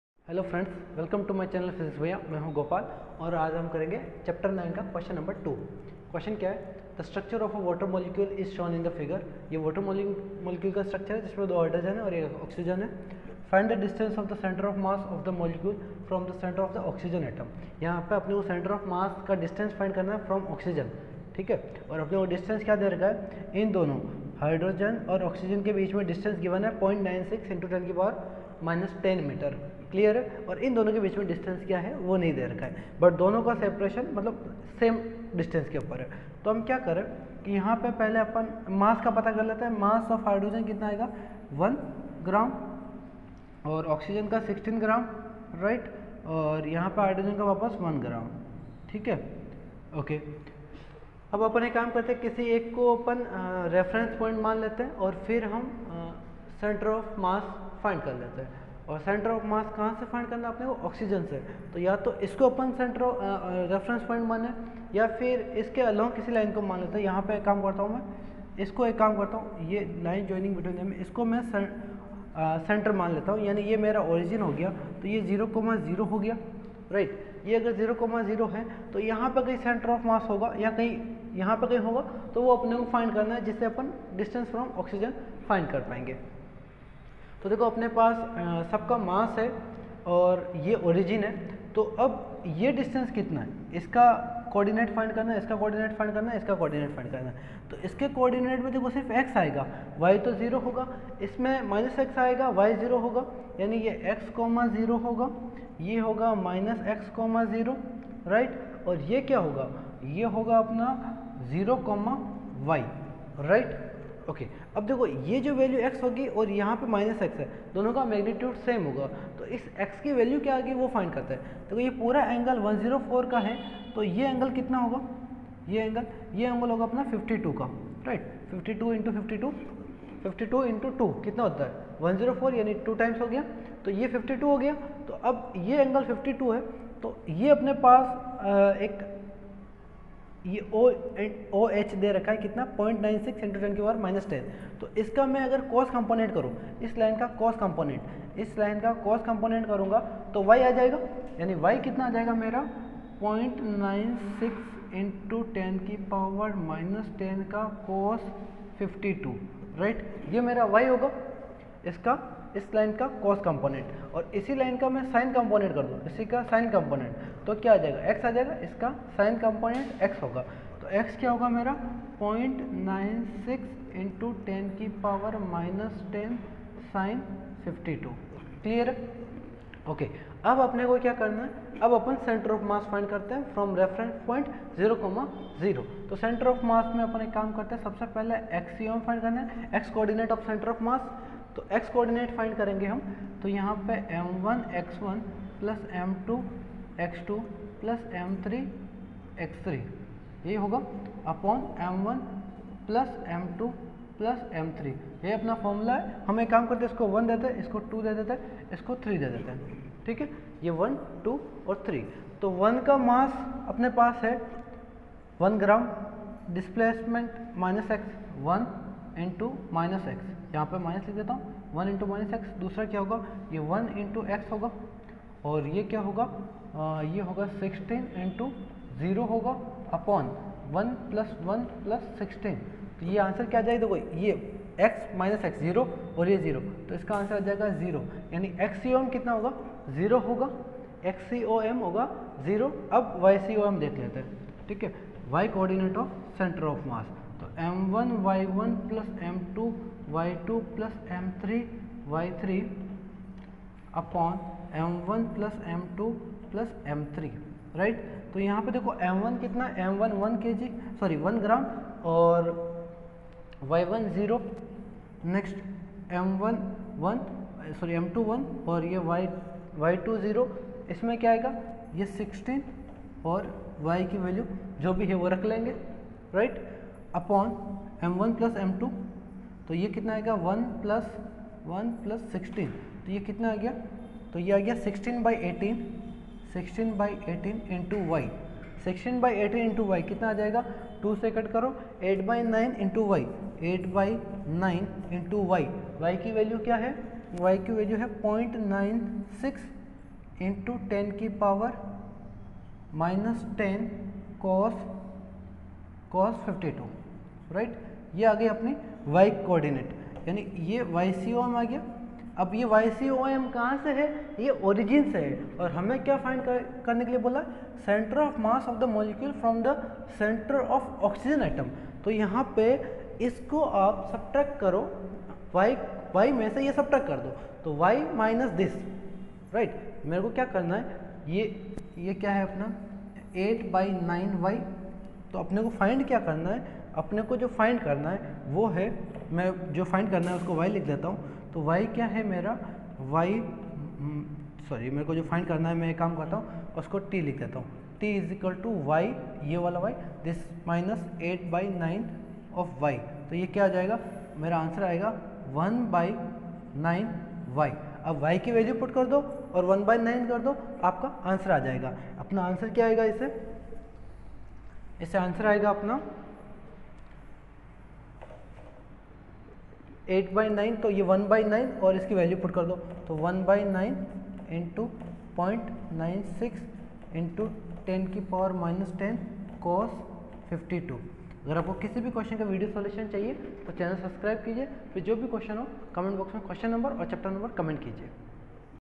H.C. Verma Solutions - centre of mass- Chapter 9, Question 2
2. The structure of a water molecule is shown in figure
(9-E1). Find the distance of the centre of mass of the
molecule from the centre of the oxygen atom.